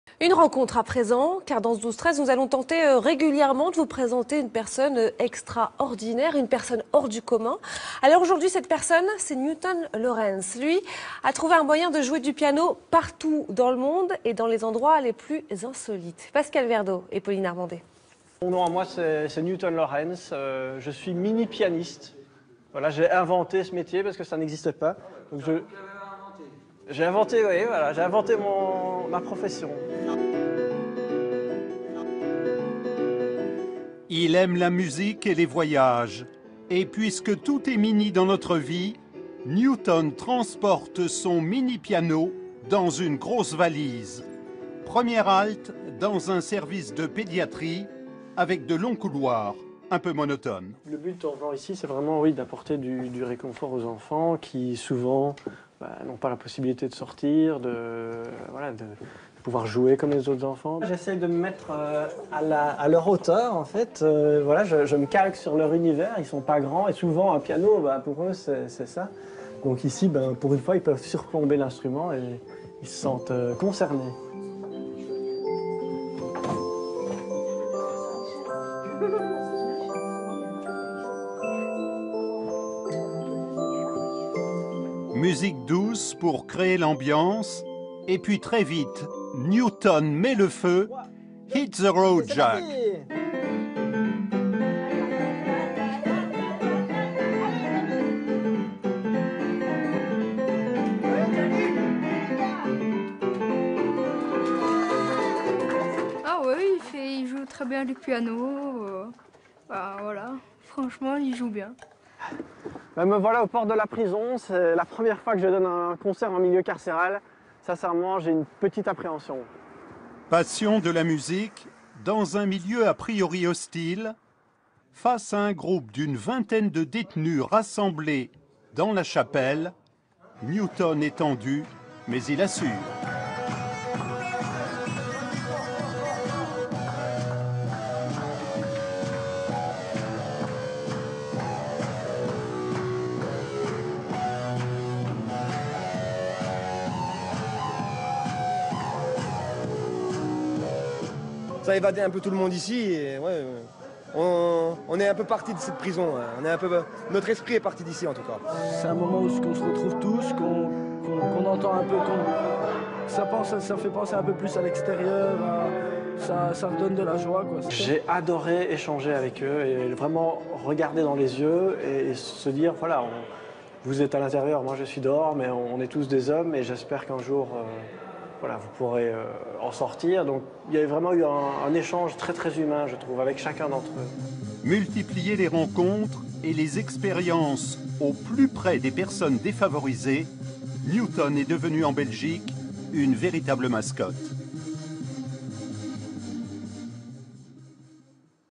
Un mini piano en milieu carcéral
Newton Lawrence, le "mini-pianiste" comme il aime être appelé, donne pour la première fois un concert en milieu carcéral. Un moment fort où les prisonniers profitent d'un moment d'évasion... en toute légalité.